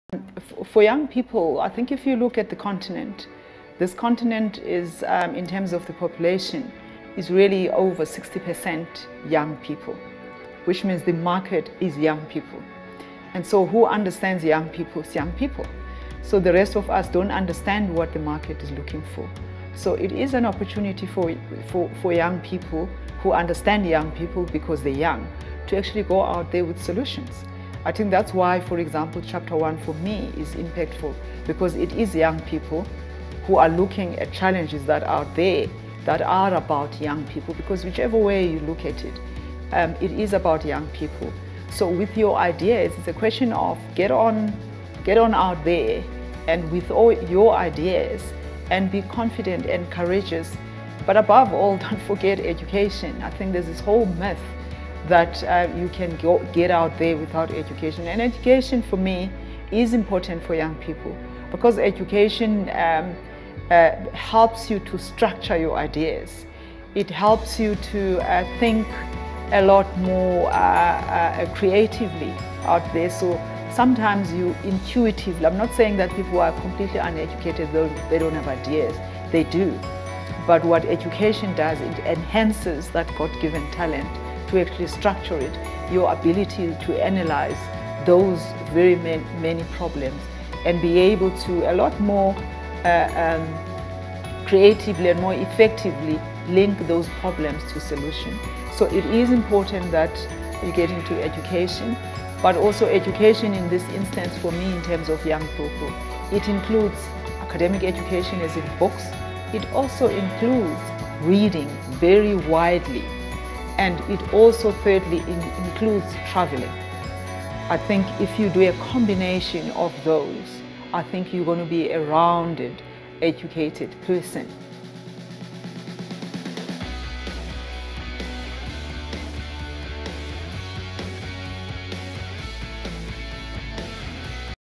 Lulu Gwagwa
Lulu Gwagwa talks about how young people should be identifying challenges that affect them and coming up with solutions for those challenges!